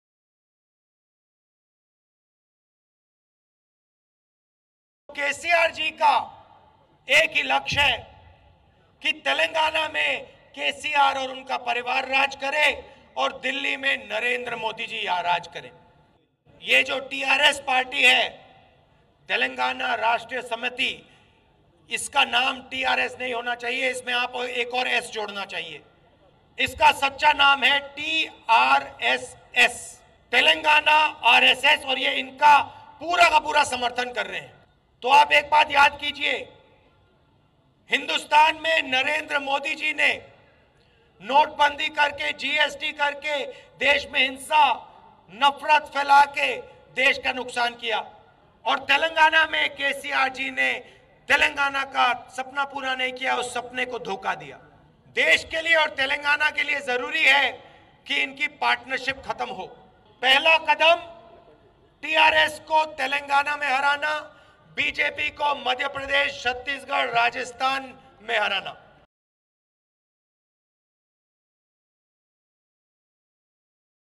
Telangana polls: TRS should be renamed TRSS, says Rahul Gandhi
Nizamabad (Telangana), Nov 29 (ANI): Congress President Rahul Gandhi on Thursday addressed a rally in Telangana’s Nizamabad where he criticised Chief Minister K Chandrashekar Rao for supporting Prime Minister Narendra Modi and therefore Rao should add another letter in his party, TRSS. While speaking at the rally, Rahul Gandhi said, “K Chandrashekar Rao's (Telangana CM) only goal is to rule Telangana and let Modi rule Delhi. There should be another letter in TRS’s name, it should be TRSS.” The state will go to polls on December 7.